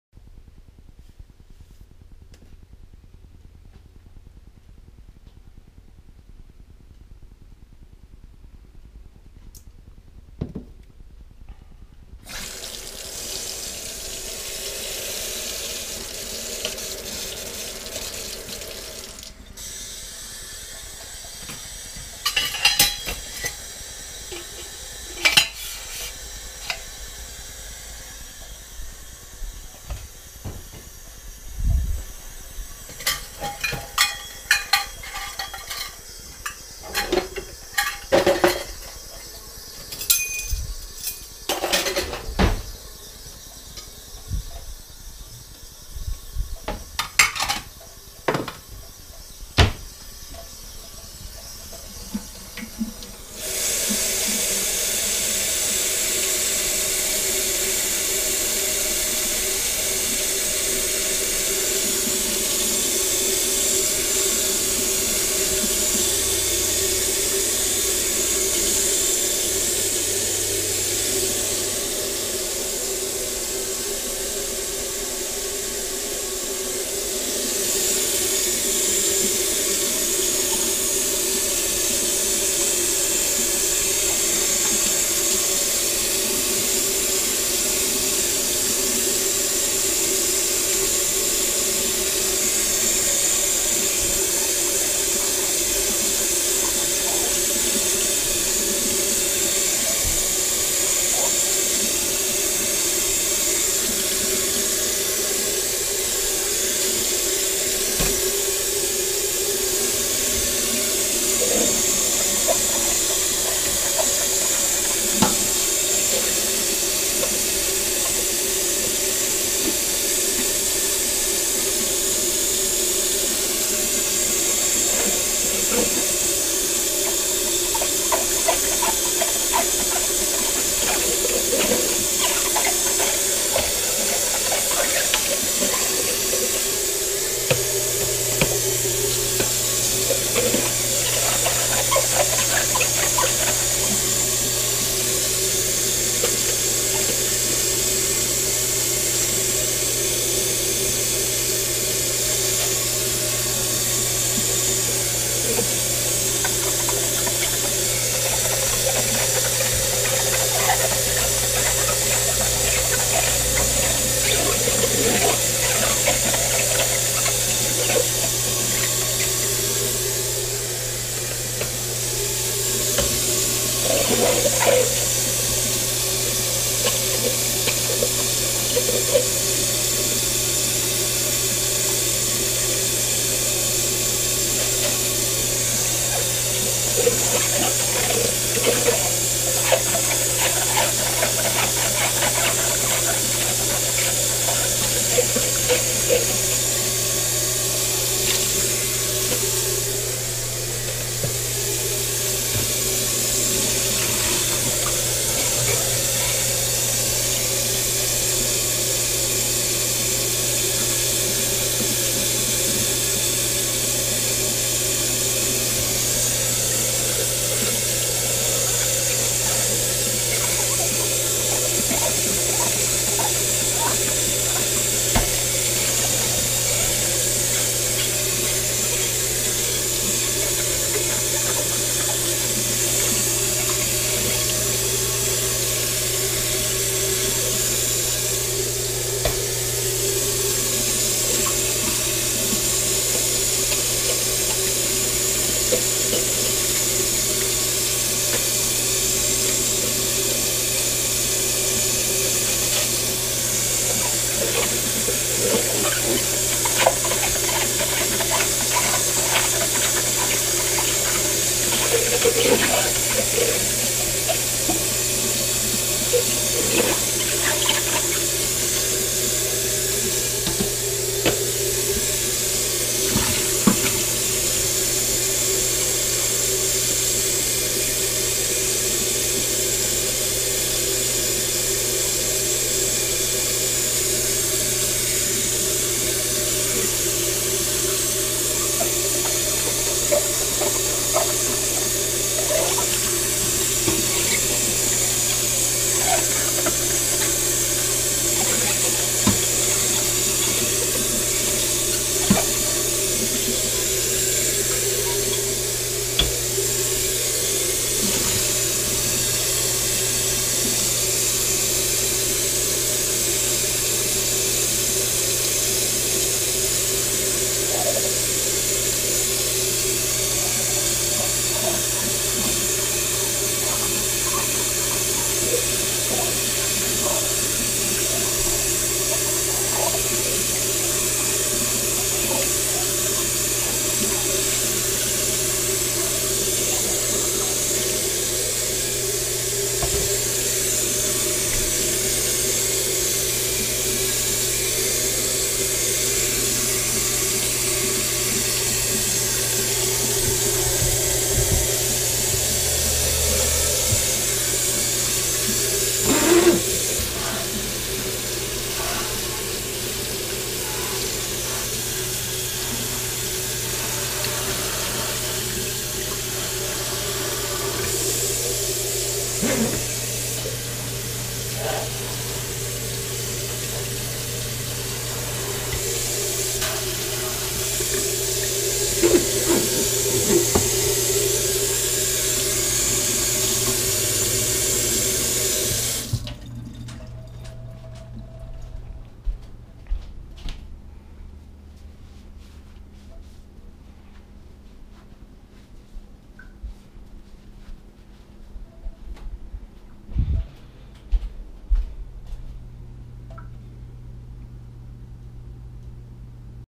DOING THE DISHES IN FIRST PERSON WOW
wow this is really dishes being done from a first person viewpoint in real time you can watch from start to finish or jump around in any order because the plot of this video is freeform and does not require you to watch any previous episodes to understand whats going on i hope you enjoy and if you don't like and subscribe this video i wish you well but probably won't respect you as much honestly